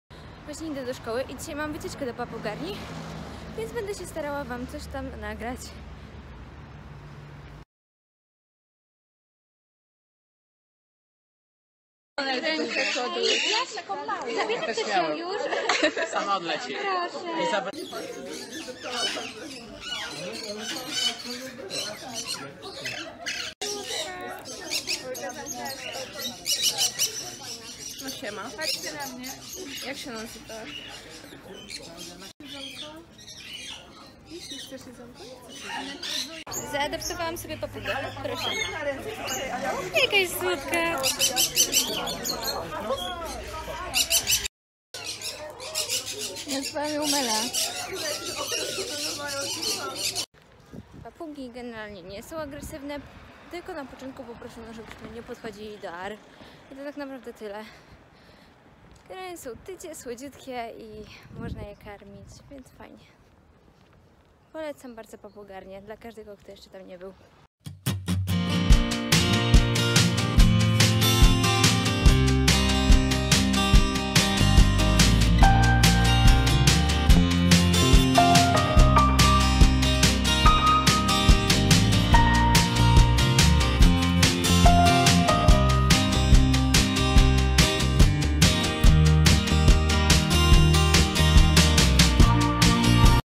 Wycieczka do Papugarni
Hejka, dzisiaj taki krótszy vlog z mojej wycieczki klasowej do Papugarni. Jak zwykle mam nadzieję,że filmik Wam się spodoba. Jeśli tak, to zostawcie łapkę w górę i miły komentarz.